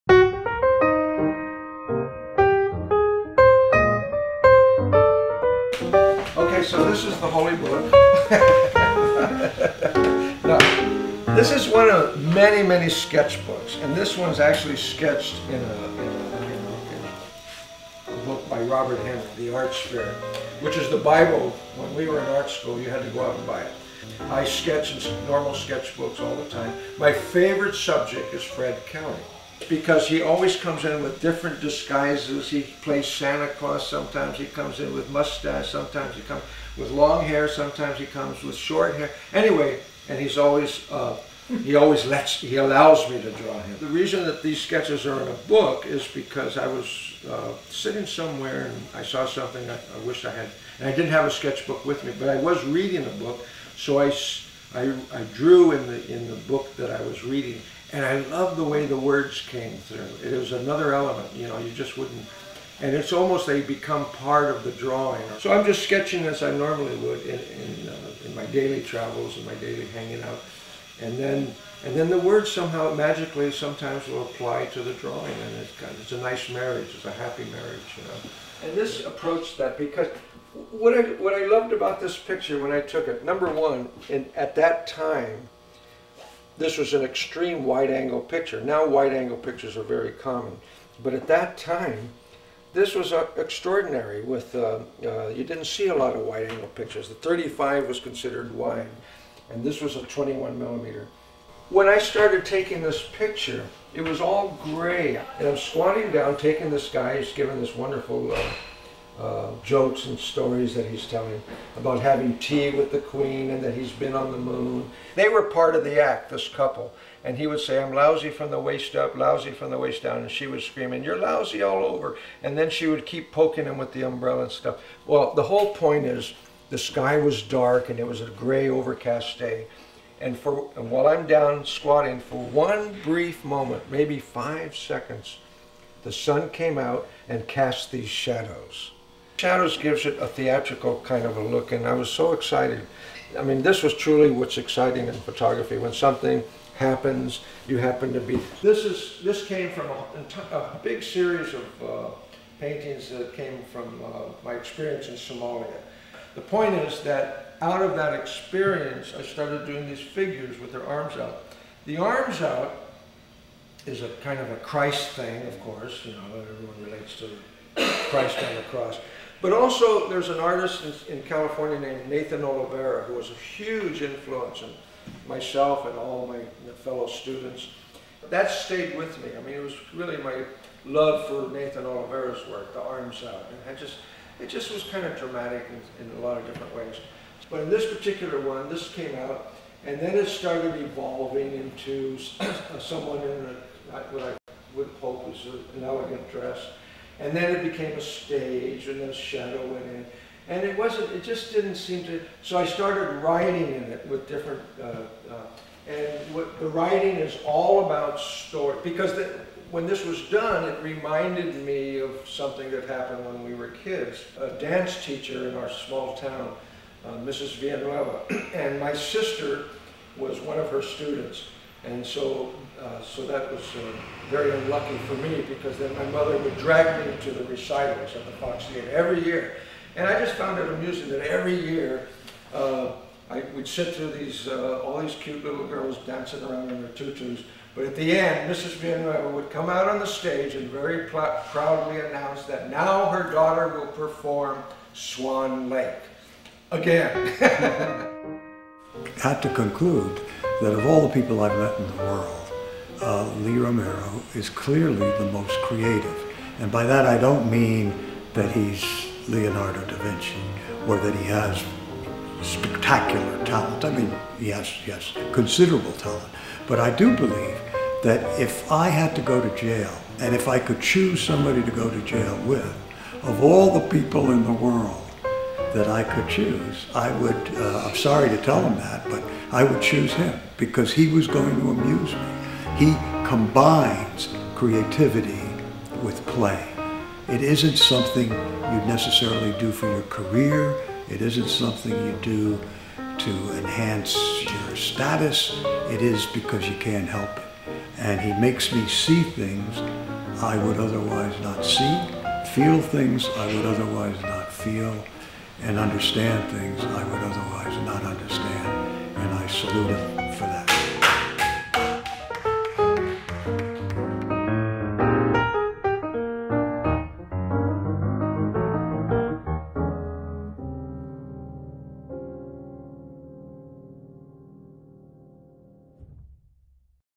Conversation with Librado Romero
Librado Romero explains his art with Michael Kaufman.